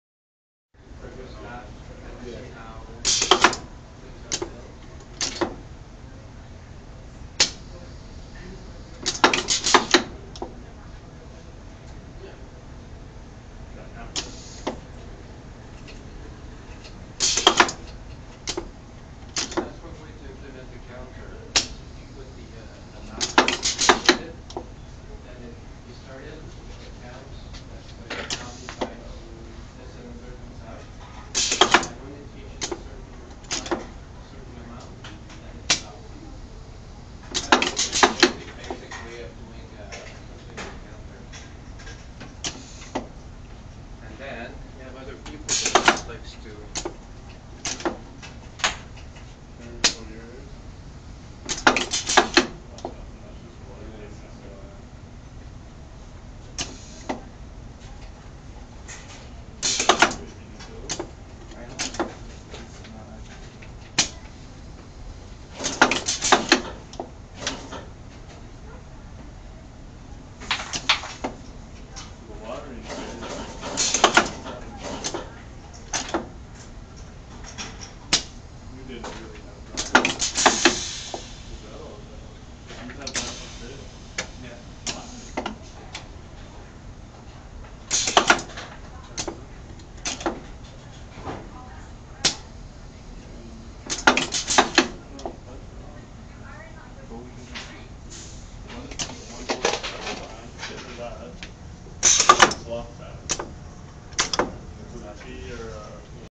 Electro-Pneumatics Final Video 3
Final project for electro-pneumatics, the system grabs a unit for packaging, prepares it then places it in a box. Once a set number have been placed in a box a large cylinder pushes the box down the line to be shipped and resets (placing another box to be filled).